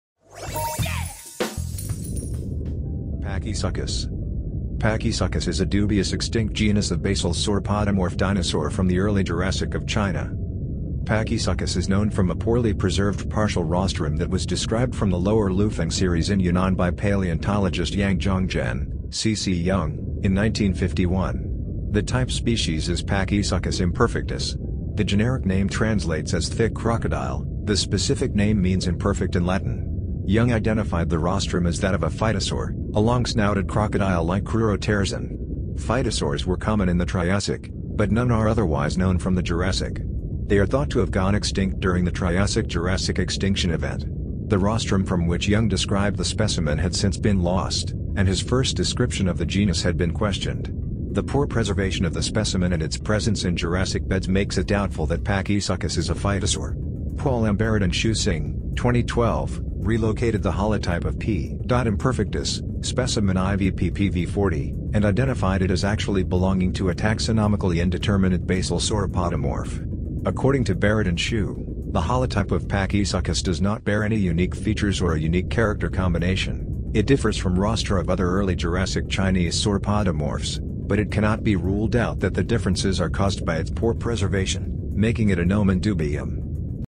Pachysuchus 🦖🌴 DINOSAURS 🌴🦖
Pachysuchus is known from a poorly preserved partial rostrum that was described from the Lower Lufeng Series in Yunnan by paleontologist Yang Zhongjian ("C.C. Young") in 1951. The type species is Pachysuchus imperfectus. The generic name translates as "thick crocodile"; the specific name means "imperfect" in Latin. Young identified the rostrum as that of a phytosaur, a long-snouted crocodile-like crurotarsan. Phytosaurs were common in the Triassic, but none are otherwise known from the Jurassic. They are thought to have gone extinct during the Triassic-Jurassic extinction event. The rostrum from which Young described the specimen had since been lost, and his first description of the genus had been questioned. The poor preservation of the specimen and its presence in Jurassic beds makes it doubtful that Pachysuchus is a phytosaur. Paul M. Barrett and Xu Xing (2012) relocated the holotype of P. imperfectus, specimen IVPP V 40, and identified it as actually belonging to a taxonomically indeterminate basal sauropodomorph. According to Barrett and Xu, the holotype of Pachysuchus "does not bear any unique features or a unique character combination"; it differs from rostra of other Early Jurassic Chinese sauropodomorphs, but it cannot be ruled out that the differences are caused by its poor preservation, making it a nomen dubium.

30 Pack Dinosaur Fossil Skeletons

Minelab GPZ 7000 Gold Nugget Metal Detector